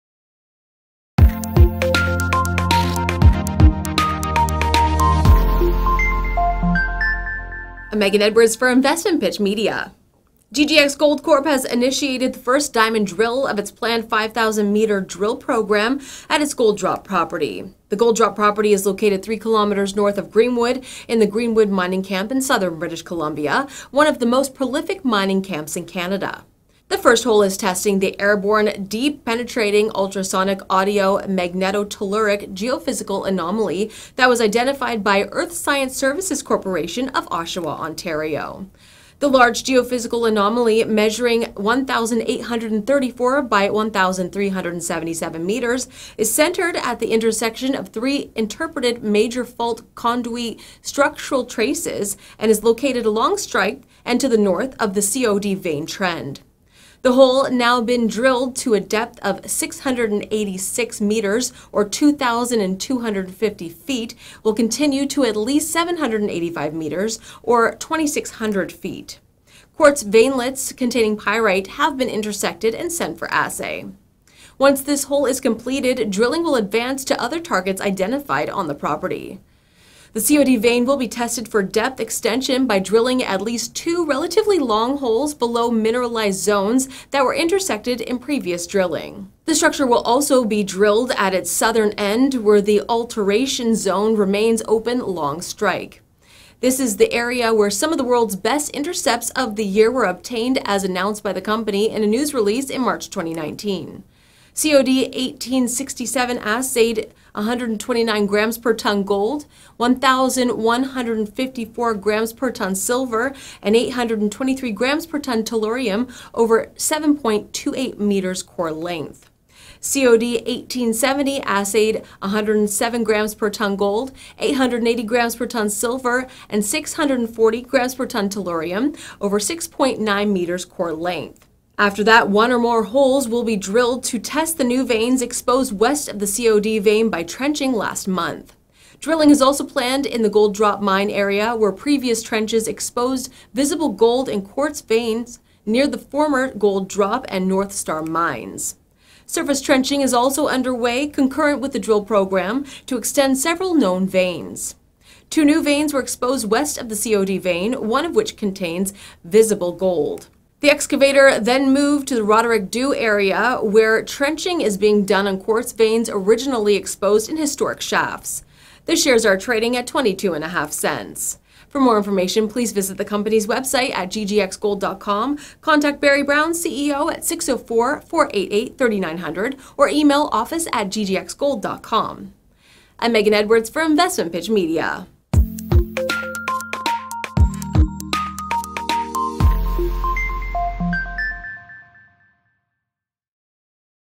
GGX Gold Expands Drill Program To 5000 Meters at Gold Drop Property in Greenwood Gold Mining Camp
The first diamond drill hole of the season is underway, testing the airborne deep-penetrating ultra-sonic AMT geophysical anomaly that was identified by Earth Science Services Corporation of Oshawa, Ontario. The hole has now been drilled to a depth of 686 metres (2,250 feet) and will be continued to at least 785 metres (2600 feet). So far, quartz veinlets containing pyrite were intersected and have been sampled for assay.